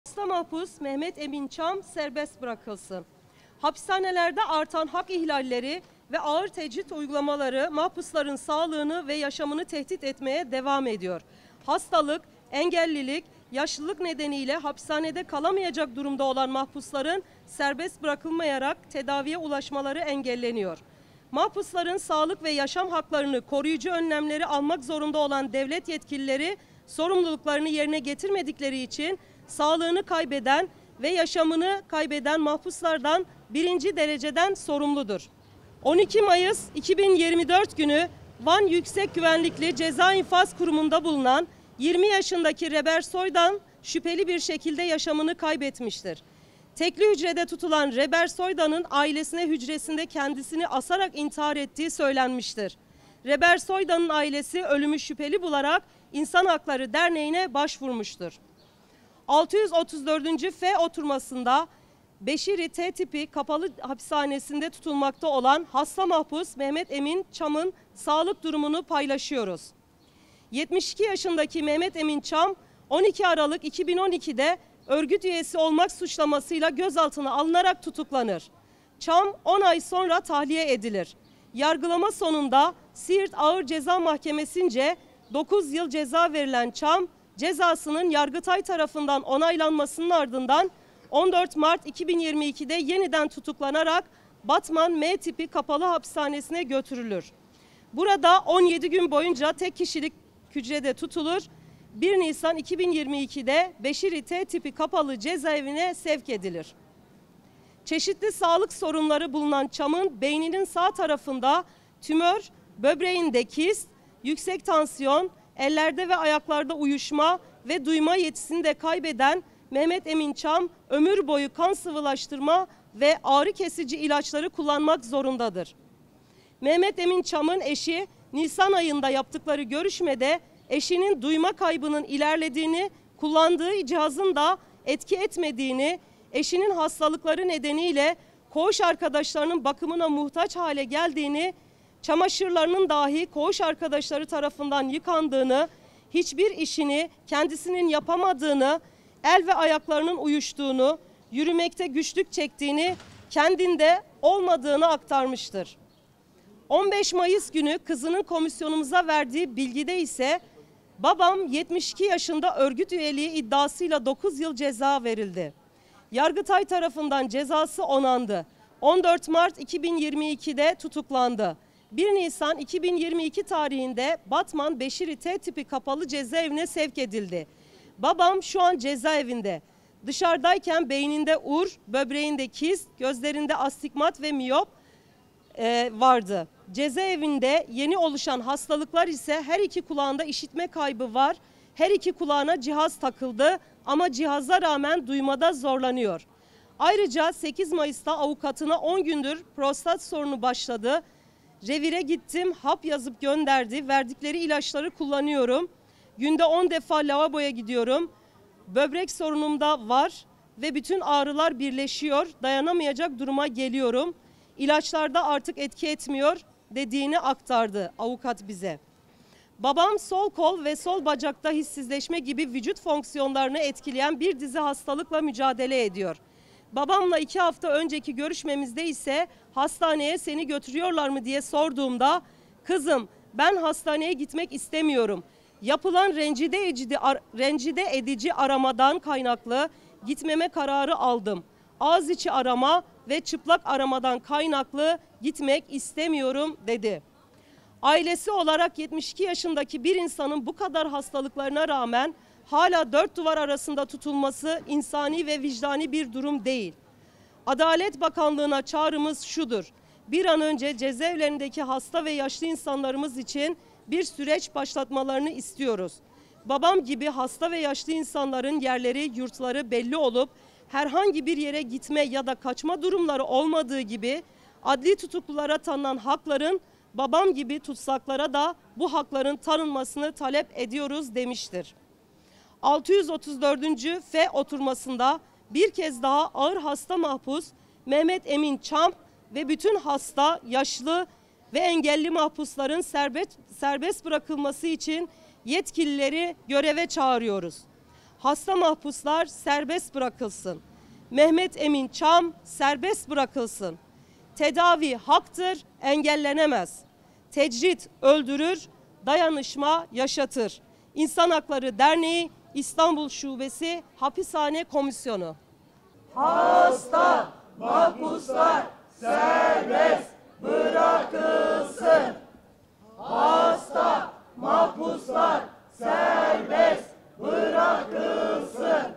634. F Oturması: Hasta mahpus Mehmet Emin Çam serbest bırakılsın
İHD İstanbul Şubesi Hapishane Komisyonu, F oturması eyleminde bu hafta halen Beşiri T Tipi Kapalı Hapishanesi’nde tutulmakta olan 72 yaşındaki hasta mahpus Mehmet Emin Çam’ın sağlık durumuna dikkat çekerek serbest bırakılmasını istedi.